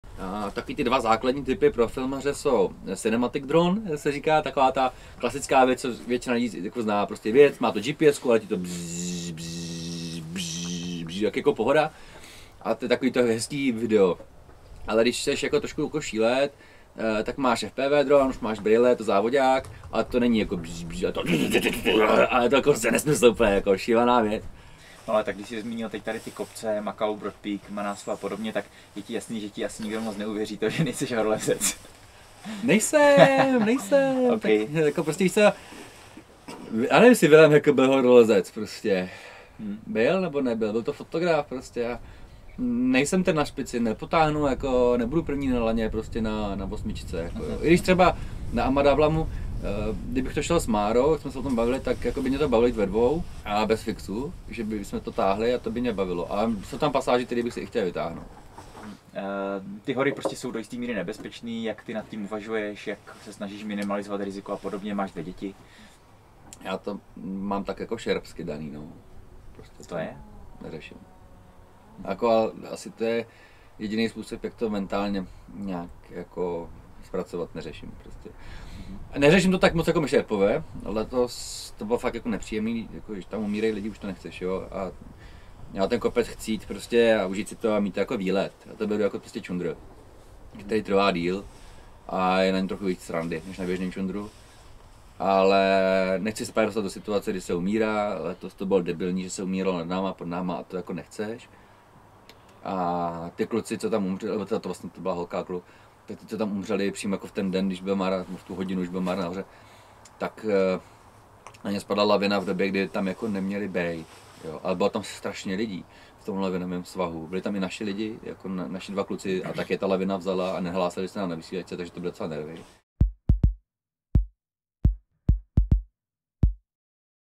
TRAILER: Fotograf a dobrodruh Petr Jan Juračka: "Nejsem horolezec. K horám přistupuju šerpsky."
Celý rozhovor s @PetrJanJuracka  najdete už brzy na naHoru.tv!
Tak náš kanál určitě odebírejte, ať vám neuteče.